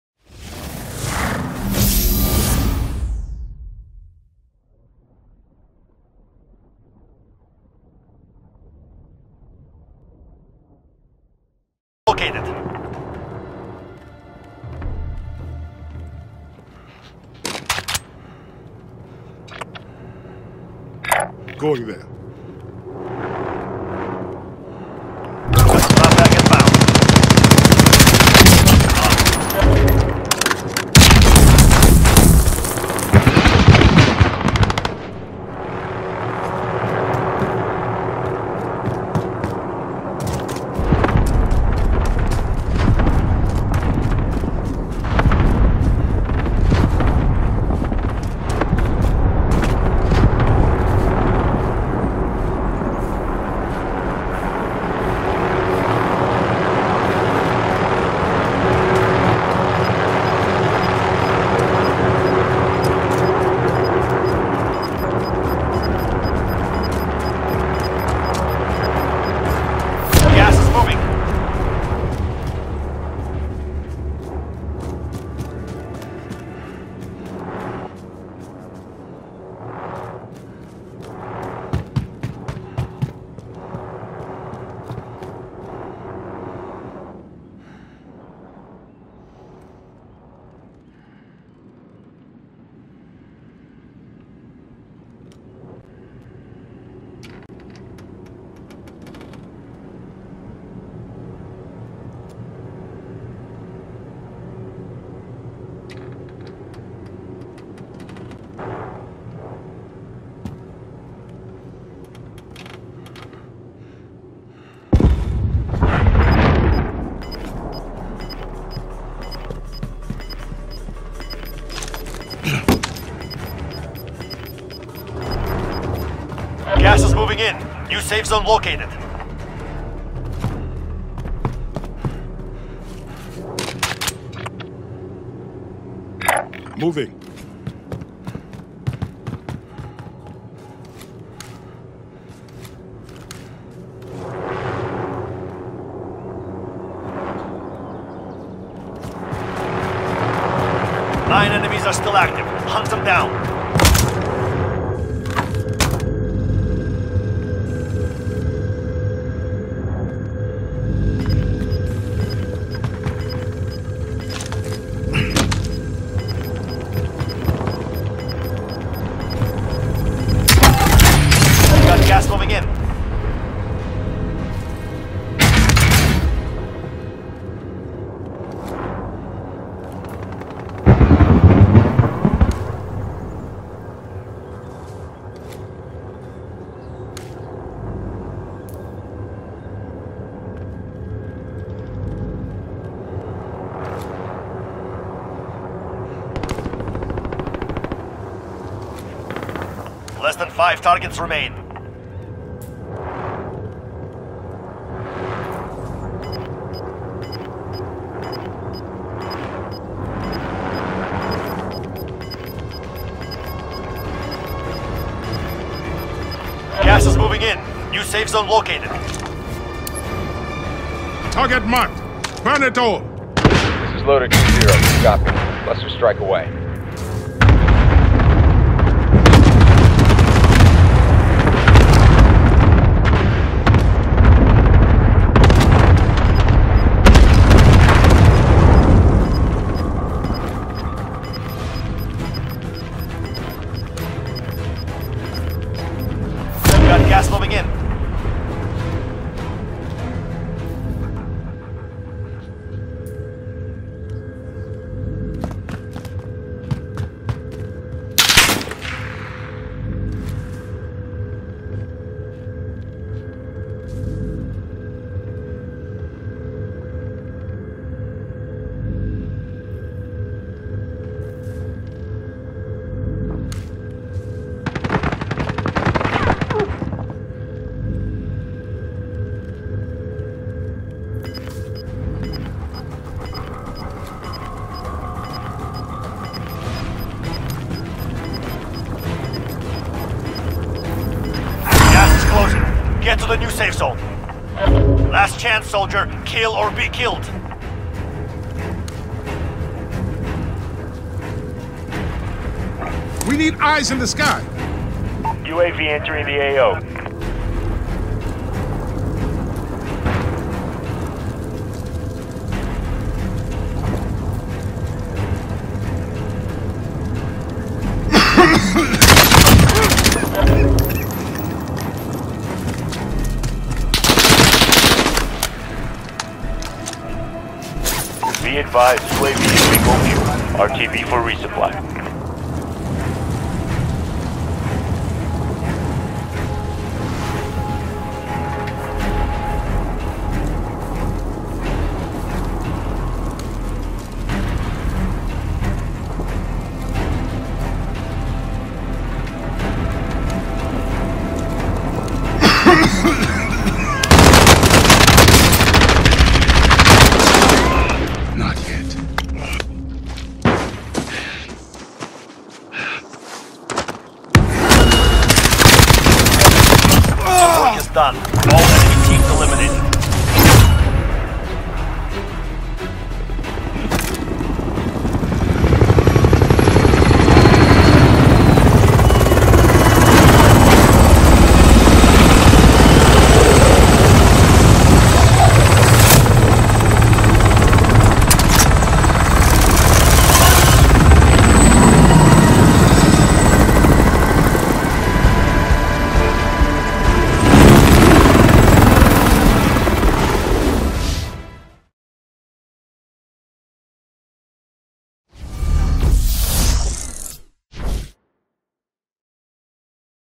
First solo game of the day. Funniest win
Revive myself against the last player. Then kill him.